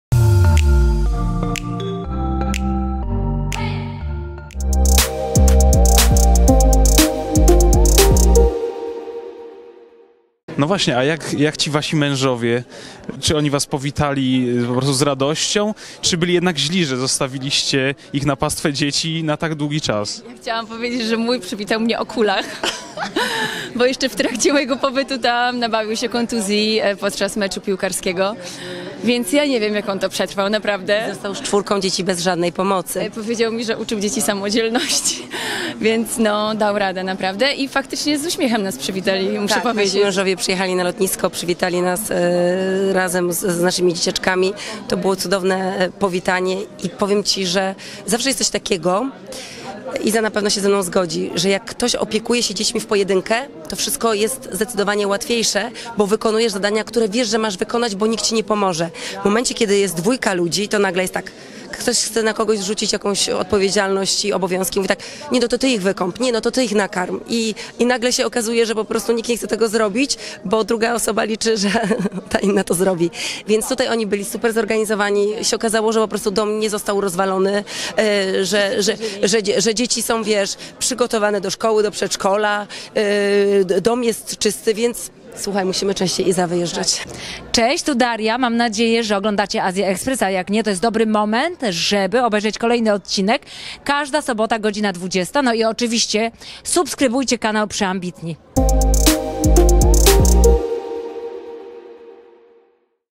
Paulina KRUPIŃSKA zostawiła męża i wyjechała do AZJI! KARPIEL-BUŁECKA poradził sobie z dziećmi?
Wywiad z Paulina Krupińska, Warszawa - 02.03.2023 r. Konfencja Azja Express